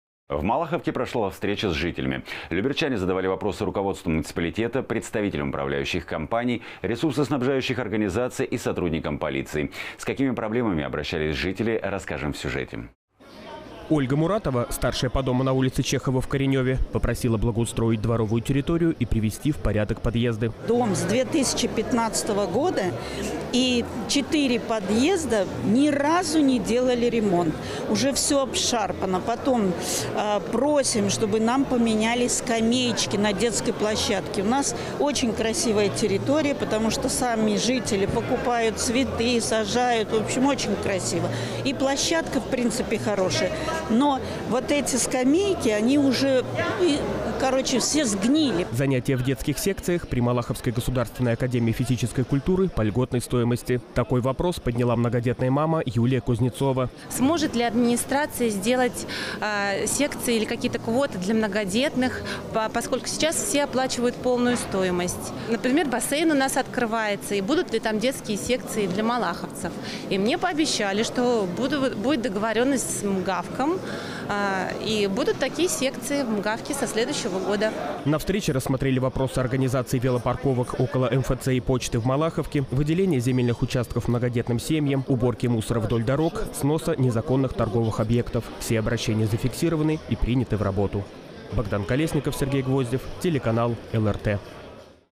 Встреча с жителями
В Малаховке прошла встреча с жителями. Люберчане задавали вопросы руководству муниципалитета, представителям управляющих компаний, ресурсоснабжающих организаций и сотрудникам полиции. С какими проблемами обращались жители, расскажем в сюжете.

Ольга Муратова – старшая по дому на улице Чехова в Коренёве – попросила благоустроить дворовую территорию и привести в порядок подъезды.

Ольга Муратова, жительница г.о. Люберцы: «Дом с 2015 года, и в четырёх подъездах ни разу не делали ремонт. Уже всё обшарпано. И просим, чтобы нам поменяли скамеечки на детской площадке. У нас очень красивая территория, потому что сами жители покупают цветы, сажают. В общем, очень красиво. И площадка, в принципе, хорошая. Но вот эти скамейки уже все сгнили».

Занятия в детских секциях при Московской государственной академии физической культуры по льготной стоимости. Такой вопрос подняла многодетная мама Юлия Кузнецова.

Юлия Кузнецова, жительница г.о. Люберцы: «Сможет ли администрация сделать секции или какие-то квоты для многодетных, поскольку сейчас все оплачивают полную стоимость. Например, бассейн у нас открывается, и будут ли там детские секции для малаховцев. Мне пообещали, что будет договорённость с МГАФКом и будут такие секции в МГАФКе со следующего года».

На встрече рассмотрели вопросы организации велопарковок около МФЦ и почты в Малаховке, выделения земельных участков многодетным семьям, уборки мусора вдоль дорог, сноса незаконных торговых объектов. Все обращения зафиксированы и приняты в работу.